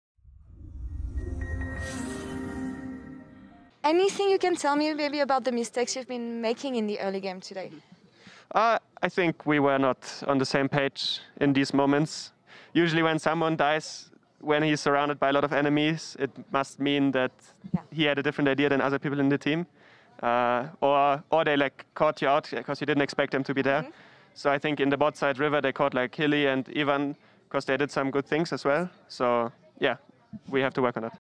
FNC Upset About Hylissang And Razork Inting In FNC vs DFM
[PLAY-INS: Day 2 - Mexico City]